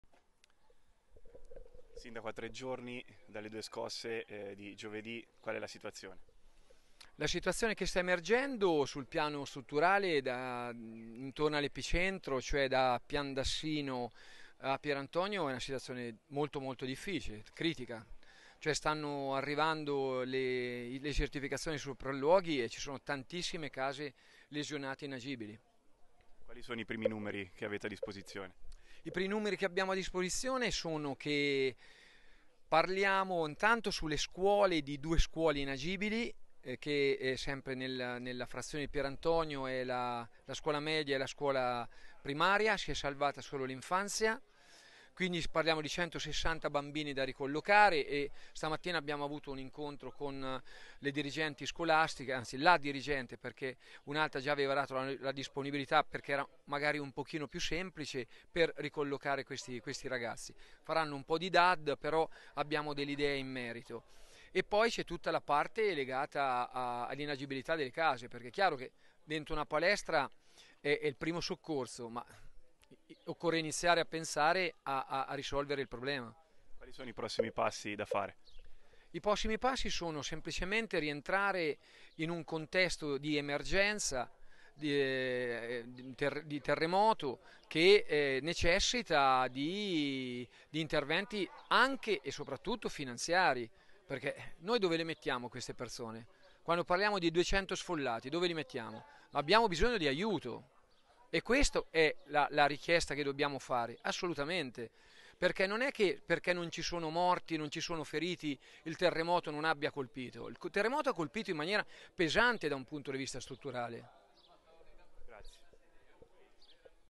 Umbertide, sindaco: «Abbiamo 200 sfollati, molte case e scuole inagibili. C’è bisogno di aiuto»
Situazione critica a Umbertide dopo le scosse di terremoto di giovedì scorso. Sono circa 200 gli sfollati come ha confermato a TeF channel il sindaco di Umbertide Luca Carizia. Molte case sopratutto nella frazione di Pierantonio risultano inagibili così come due scuole che portano alla necessità di ricollocare più di 160 bambini.